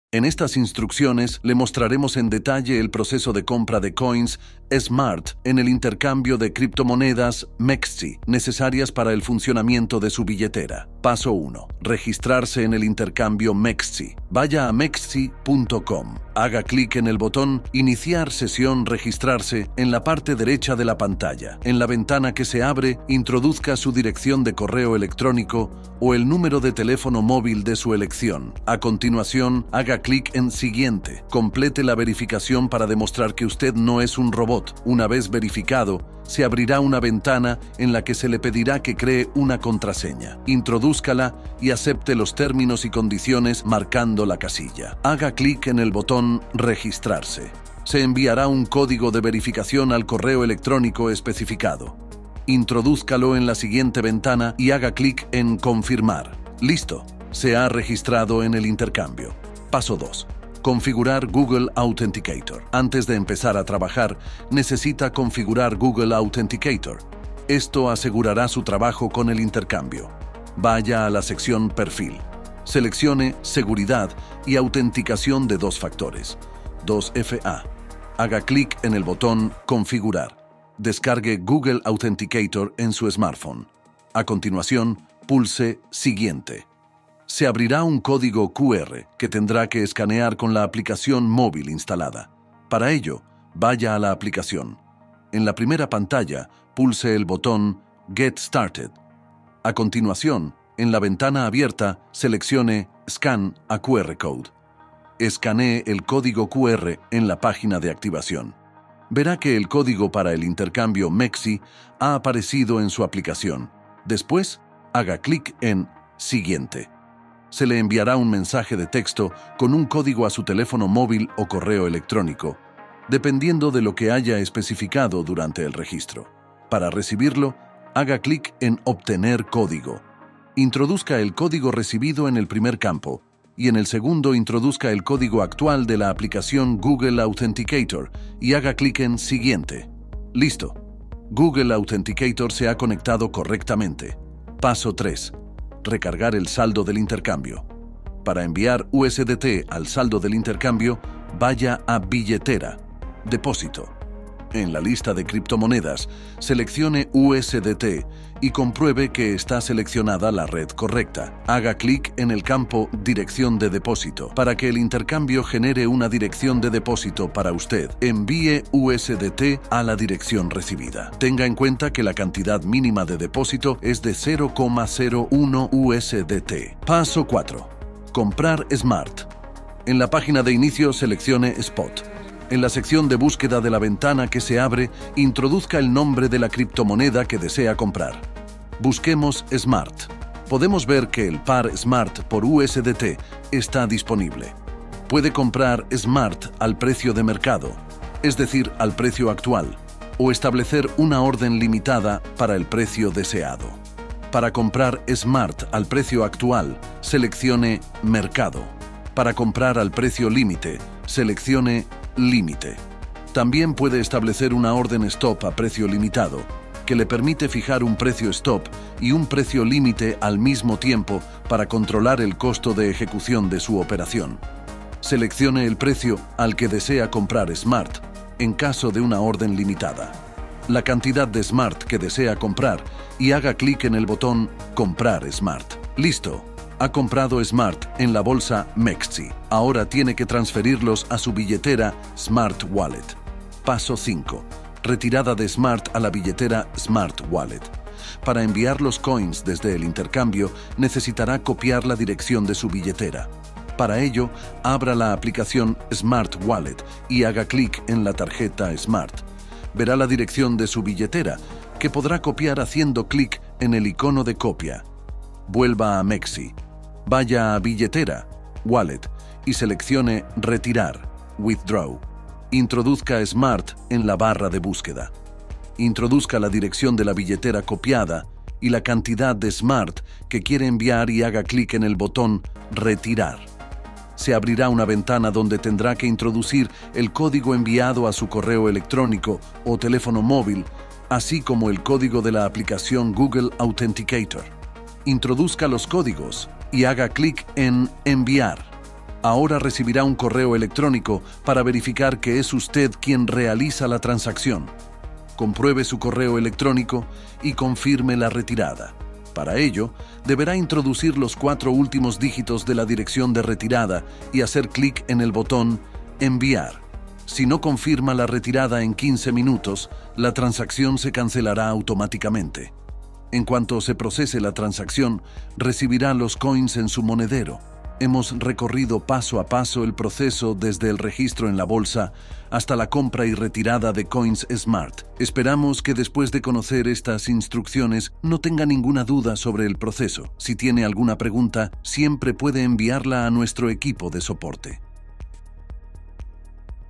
Cómo adquirir el coin SMART en MEXC: registro, KYC y compra
En esta guía, le mostramos detalladamente el proceso de compra del coin SMART en la plataforma de intercambio de criptomonedas MEXC. SMART es esencial para el funcionamiento de su billetera.

Para ello, debe seguir 5 pasos:
☑️ Registrarse en la plataforma MEXC
☑️ Configurar Google Authenticator
☑️ Recargar el saldo en la plataforma
☑️ Comprar SMART
☑️ Transferir SMART a la billetera SMART Wallet